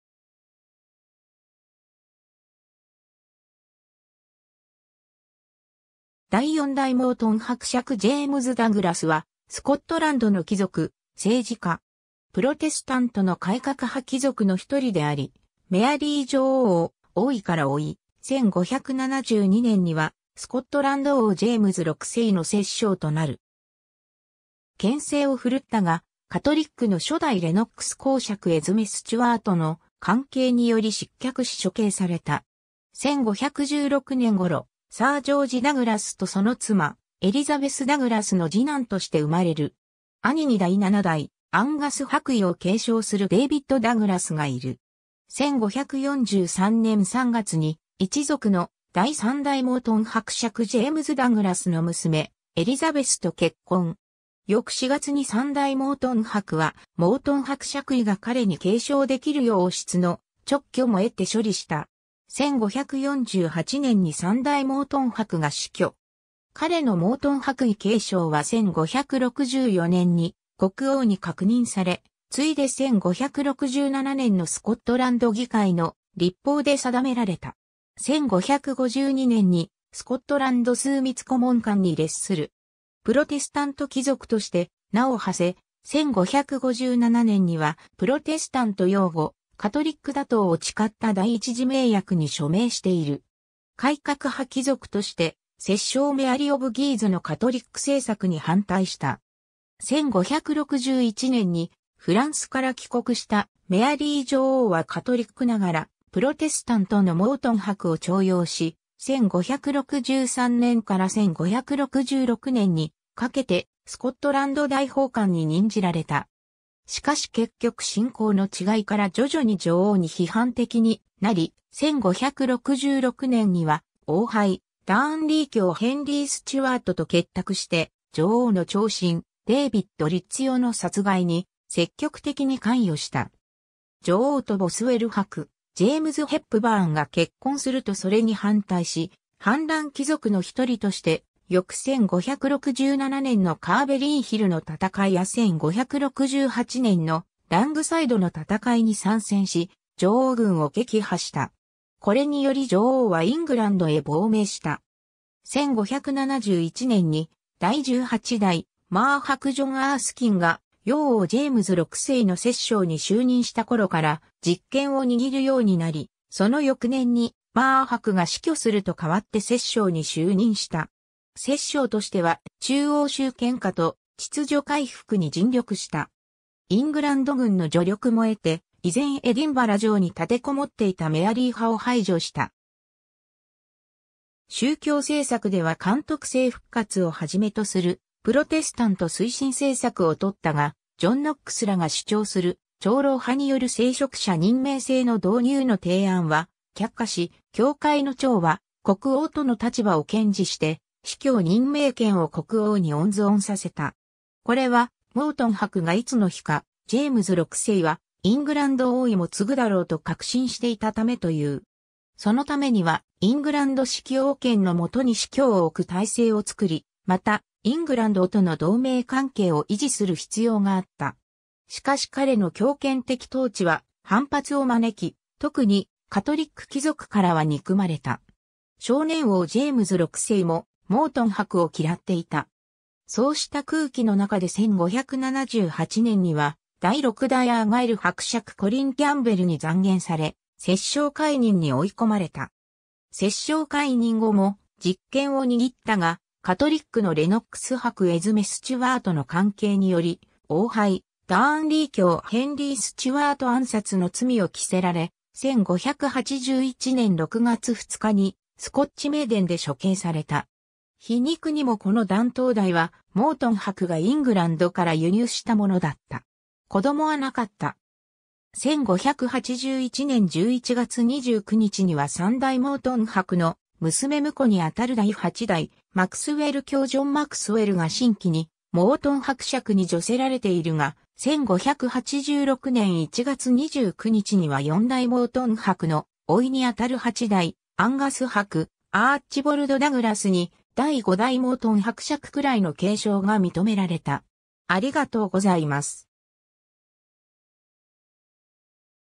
ジェイムズ・ダグラス (第4代モートン伯)
第4代モートン伯爵ジェイムズ・ダグラス（英: James Douglas, 4th Earl of Morton、1516年頃 - 1581年6月2日）は、スコットランドの貴族・政治家。
 プロテスタントの改革派貴族の一人であり、メアリー女王を王位から追い、1572年にはスコットランド王ジェイムズ6世の摂政となる。
権勢をふるったが、カトリックの初代レノックス公爵エズメ・ステュワートの奸計により失脚し処刑された。
 1516年頃、サー・ジョージ・ダグラス(第5代アンガス伯アーチボルド・ダグラスの孫)とその妻エリザベス・ダグラスの次男として生まれる。
兄に第7代アンガス伯位を継承するデイヴィッド・ダグラスがいる。
 1543年3月に一族の第3代モートン伯爵ジェイムズ・ダグラスの娘エリザベスと結婚。
翌4月に3代モートン伯はモートン伯爵位が彼（その次に兄の7代アンガス伯）に継承できるよう王室の勅許も得て処理した。
 1548年に3代モートン伯が死去。
彼のモートン伯位継承は1564年に国王に確認され、ついで1567年のスコットランド議会の立法で定められた。
 1552年にスコットランド枢密顧問官に列する。
プロテスタント貴族として名を馳せ、1557年にはプロテスタント擁護・カトリック打倒を誓った第一次盟約(First Bond)に署名している。
改革派貴族として摂政メアリ・オブ・ギーズのカトリック政策に反対した。
 1561年にフランスから帰国したメアリー女王はカトリックながらプロテスタントのモートン伯を重用し、1563年から1566年にかけてスコットランド大法官に任じられた。
しかし結局信仰の違いから徐々に女王に批判的になり、1566年には王配ダーンリー卿ヘンリー・ステュワートと結託して女王の寵臣デイヴィッド・リッツィオの殺害に積極的に関与した。
 女王とボスウェル伯ジェームズ・ヘップバーンが結婚するとそれに反対し、反乱貴族の一人として翌1567年のカーベリーヒルの戦いや1568年のラングサイドの戦いに参戦し、女王軍を撃破した。
これにより女王はイングランドへ亡命した（後にイングランドで処刑される）。
 1571年に第18代マー伯ジョン・アースキンが幼王ジェームズ6世の摂政に就任した頃から実権を握るようになり、その翌年にマー伯が死去すると代わって摂政に就任した。
 摂政としては中央集権化と秩序回復に尽力した。
イングランド軍の助力も得て、依然エディンバラ城に立てこもっていたメアリー派を排除した。
 宗教政策では監督制(Episcopacy)復活をはじめとするプロテスタント推進政策をとったが、ジョン・ノックスらが主張する長老派による聖職者任命制の導入の提案は却下し、「教会の長は国王」との立場を堅持して司教任命権を国王に温存させた。
これはモートン伯がいつの日かジェームズ6世はイングランド王位も継ぐだろうと確信していたためという。
そのためにはイングランド式王権のもとに司教を置く体制を作り、またイングランドとの同盟関係を維持する必要があった。
 しかし彼の強権的統治は反発を招き、特にカトリック貴族からは憎まれた。
少年王ジェームズ6世もモートン伯を嫌っていた。
そうした空気の中で1578年には第6代アーガイル伯爵コリン・キャンベルに讒言され、摂政解任に追い込まれた。
 摂政解任後も実権を握ったが、カトリックのレノックス伯エズメ・ステュワート(後の初代レノックス公爵)の奸計により、王配ダーンリー卿ヘンリー・ステュワート暗殺の罪を着せられ、1581年6月2日にスコッチ・メイデン（ギロチン）で処刑された。
皮肉にもこの断頭台はモートン伯がイングランドから輸入したものだった。
 子供はなかった。
1581年11月29日には3代モートン伯の娘婿にあたる第8代マクスウェル卿ジョン・マクスウェルが新規にモートン伯爵に叙せられているが、1586年1月29日には4代モートン伯の甥にあたる8代アンガス伯アーチボルド・ダグラスに第5代モートン伯爵位の継承が認められた。